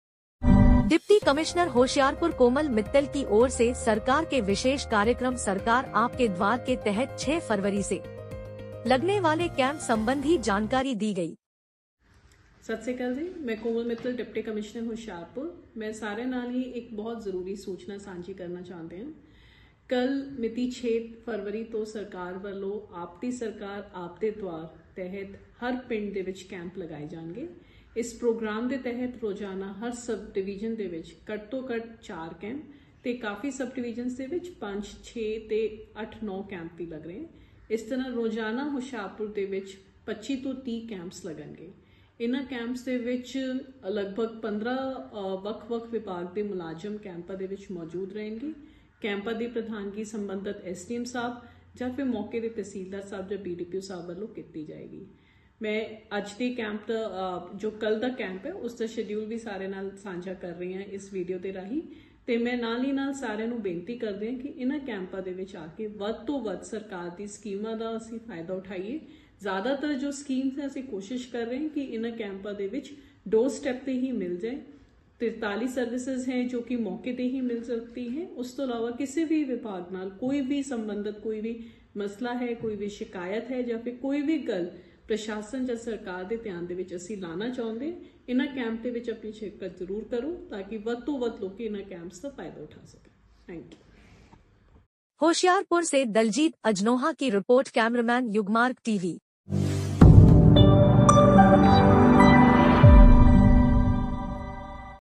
सरकार आप के द्वार के तहत फरवरी से लगने वाले कैंप संबंधी जानकारी दी गई YUGMARGTV
डिप्टी कमिश्नर होशियारपुर कोमल मित्तल की ओर से सरकार के विशेष कार्यक्रम सरकार आप के द्वार  के  तहत 6 फरवरी से  लगने वाले कैंप संबंधी जानकारी दी गई